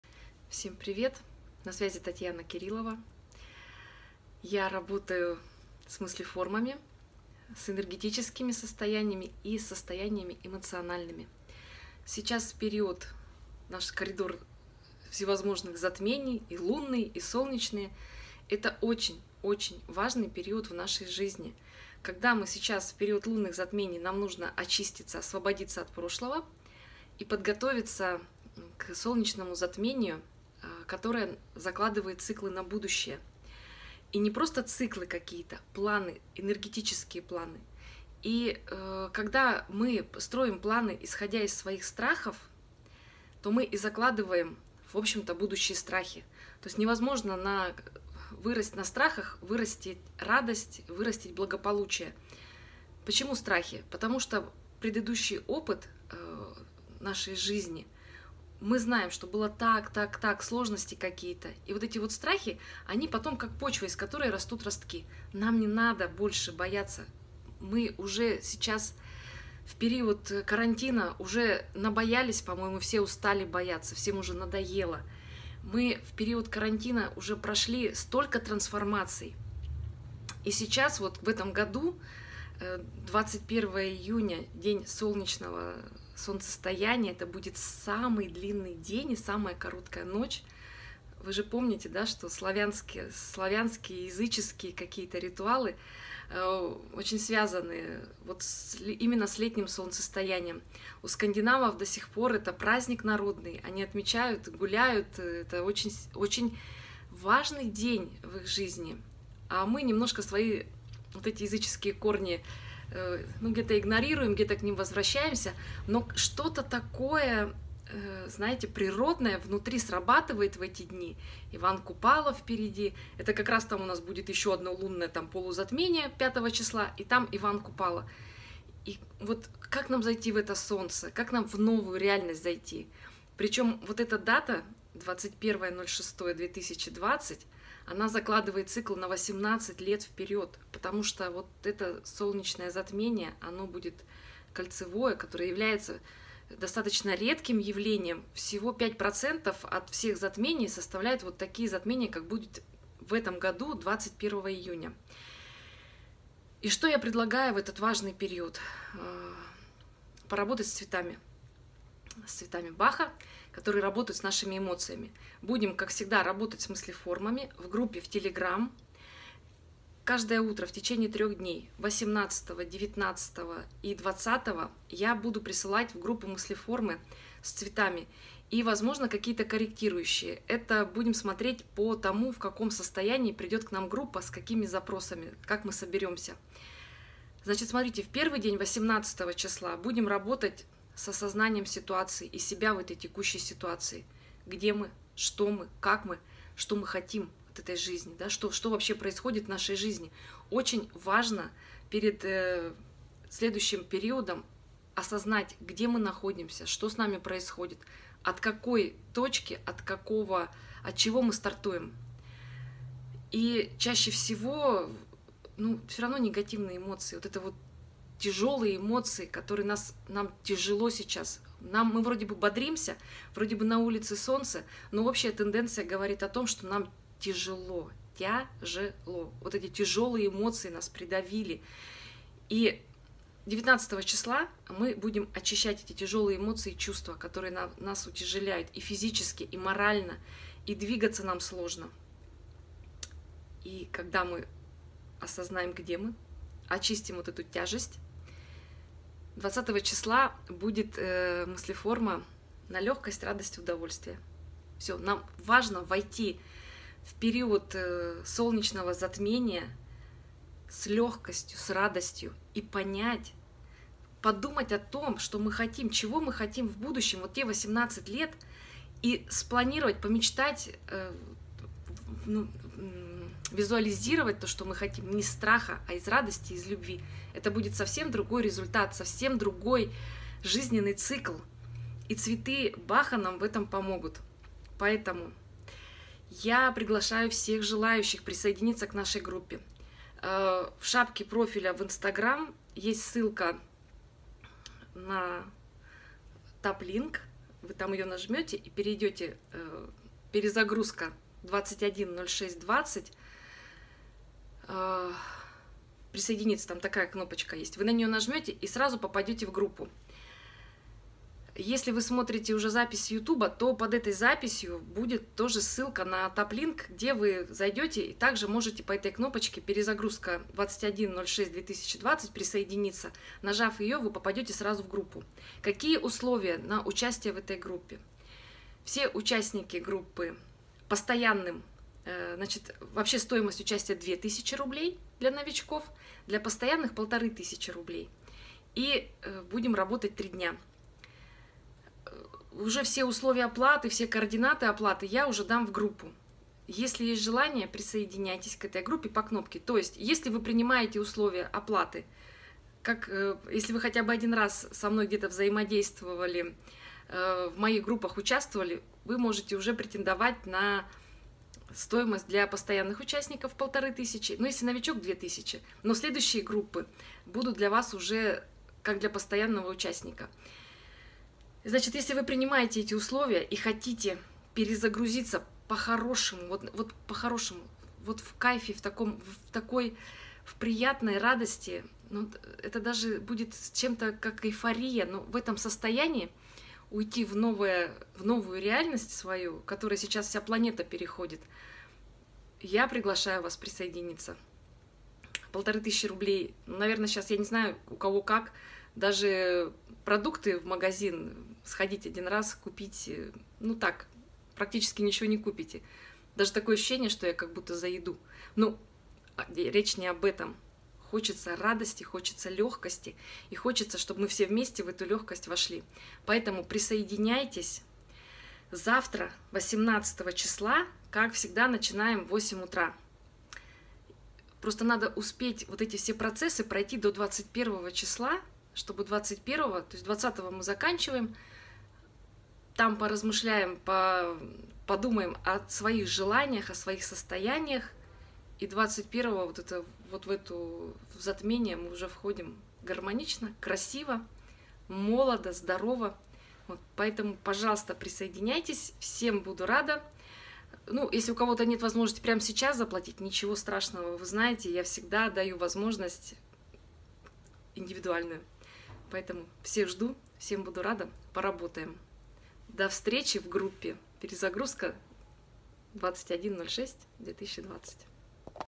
Кирилова Татьяна. Перезагрузка 21.06.2020
Готовимся к очень важной астрологической дате 21 июня.

День летнего солнцестояния – это сам по себе очень важный день, самый длинный день в году и самая короткая ночь. Это очень мощный по энергетике период.
А в этом году в это день произойдет кольцеобразное затмение Солнца. Это самый редкий вид затмения. Оно подчеркивает начало нового, происходит перезагрузка всех энергетических процессов на Земле.
Лунные затмения – это завершение периодов, работа с прошлым. Солнечные затмения – это всегда взгляд в будущее, зарождение чего-то нового.
Поэтому именно сейчас важно произвести перезагрузку, оставить все старое отболевшее прошлому, и с новыми силами и идеями двинуться вперед.
В этом году как никогда прежде, важно сделать выбор своего будущего осознанно. Если последнее время преобладал стресс, страхи и негативные эмоции, то выбор будущего может быть сформирован на базе проблем и переживаний.
Выбор сделанный от страха всегда приводит к повторению уроков и даже к их усугублению. Именно поэтому нужно войти в перезагрузку в гармоничном состоянии.
Что я предлагаю в этот очень важный период, который закладывает 18-ти летние циклы.
Поработать с цветами, но не совсем традиционно.
Обычно мы с помощью цветов восстанавливаем эмоциональное состояние. В этот раз будет 3 дня работ с цветами:
18 июня Осознание ситуации, себя в текущей ситуации.
19 июня Очищение от тяжелых эмоций и чувств
20 июня Легкость, радость, удовольствие.
Как обычно, начнем в группе в Телеграм. Стоимость участия 2 тысячи рублей для новичков и 1,5 тысячи для постоянных участников.
Чтоб принять участие в группе «Перезагрузка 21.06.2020»